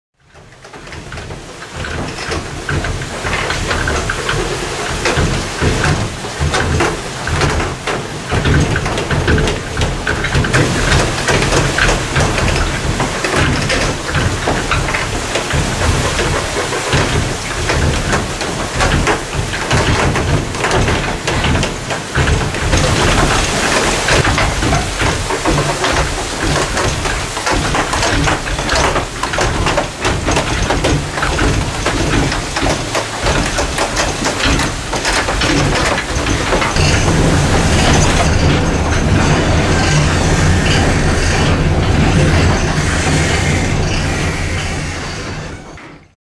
Floating mill – grinding stones (Island of Love)
Sound of the floating mill in operation (grinding stones).
The mill is mounted on two interconnected floating platforms (boats) and can be moved up or downstream. The millstones are driven by a waterwheel via a transmission shaft. The whole floating structure is tethered to the riverbank by chains.
The mill is located on the branch of the Mura River on the "Island of Love" near Ižakovci. It was built in 1999 to the original plans of a ship mill dating from the early 20th century. Such mills were gradually replaced by those powered by electricity, making this the only operational mill in Slovenia which is fully in the water.
A peculiarity of the Pannonian basin, floating mills were either partially or fully floated on a river. These types of mill were developed because of the frequent changes in level of the Pannonian watercourses, and so can readily adjust to the varying levels of these tidal rivers. Today, they only operate in the north-eastern part of Slovenia on the Mura River.